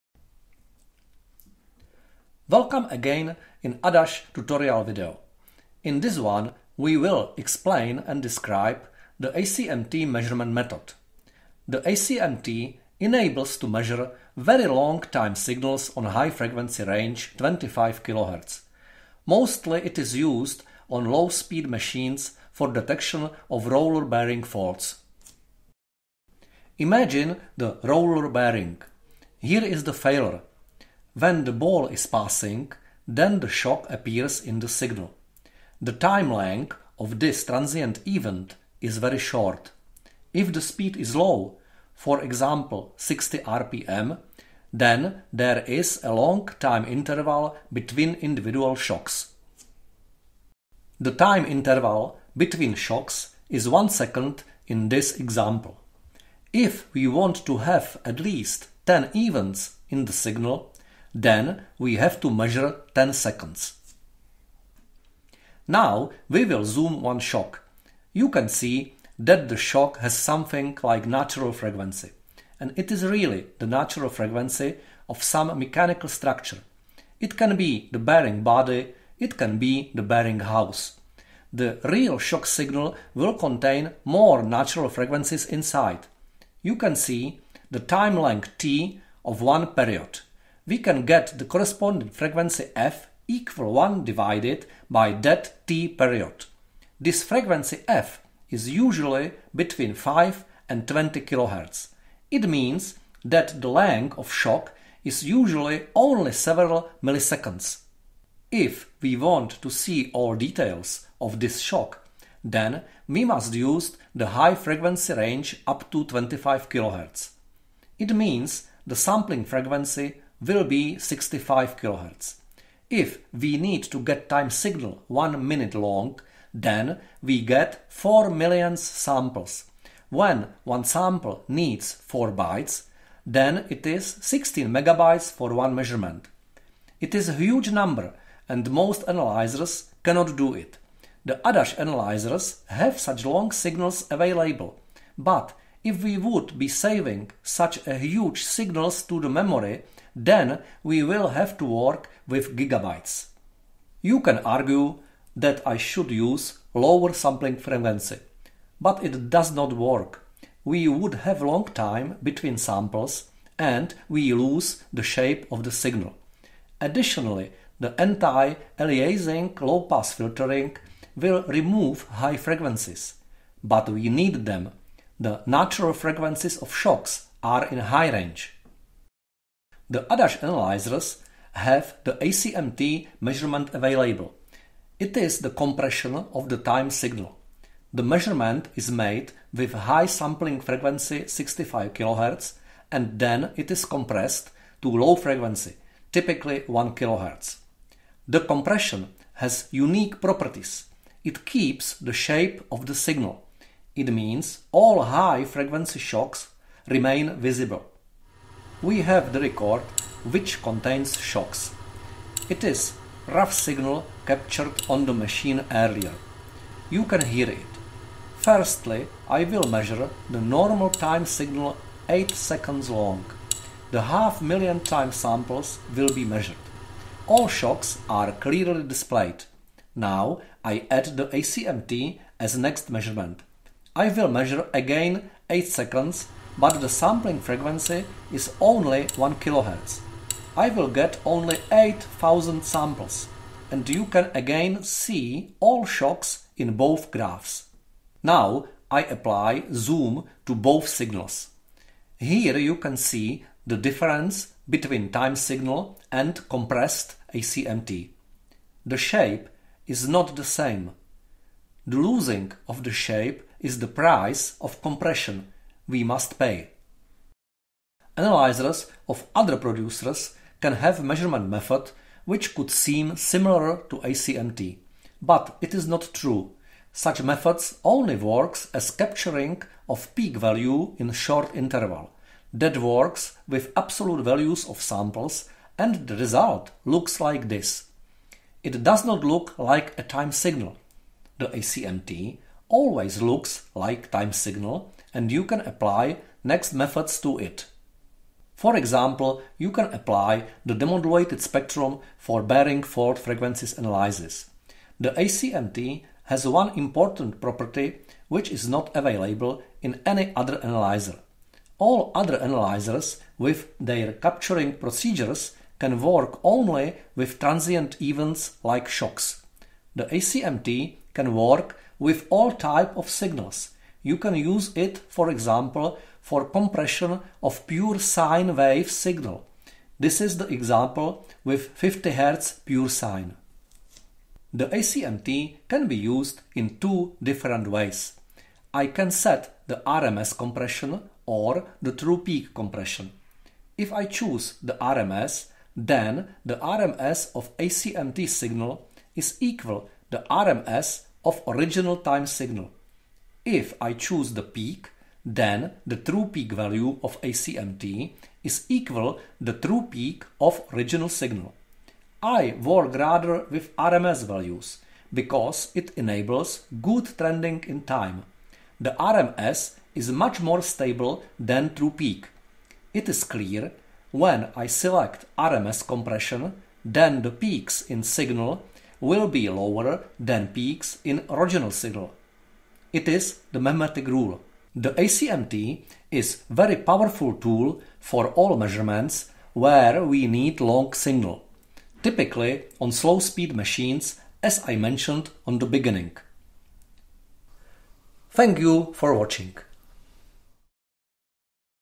ACMT - Low Speed Machinery and Bearing Analysis
This video describes our ACMT measurement method (Adash Compressed Measurement Time) which is used for low speed machinery / bearing analysis.